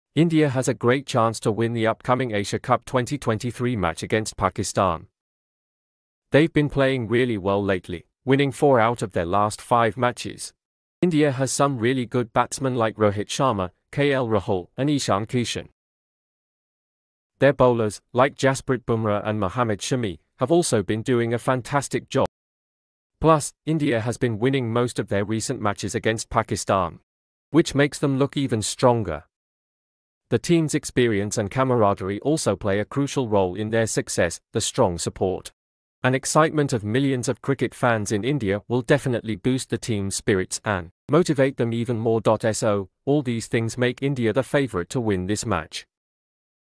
India Vs Pakistan | Asia World Cup 2023
India's cricket team is gearing up for an exciting showdown in the upcoming Asia Cup 2023 match against arch-rivals Pakistan. 🇮🇳 With an impressive track record of winning four out of their last five matches, India is riding high on their recent successes.

Their batting prowess, featuring stalwarts like Rohit Sharma, KL Rahul, and the emerging star Ishan Kishan, has been delivering impressive runs, setting the stage for an electrifying performance.

On the bowling front, the likes of Jasprit Bumrah and Mohammed Shami have been exceptional, adding to India's formidable strength.

Moreover, India's history of recent victories over Pakistan adds to their confidence. 🥇

But it's not just talent; it's the relentless hard work, dedication, and the tight-knit spirit of the team that propel India forward. Their unity and experience make them a force to reckon with. 💪

And let's not forget the millions of passionate cricket fans across India whose support and enthusiasm will undoubtedly provide an extra dose of motivation. 📢

India's path to triumph seems illuminated, and fans can't wait to see them shine in this highly-anticipated match. 🌟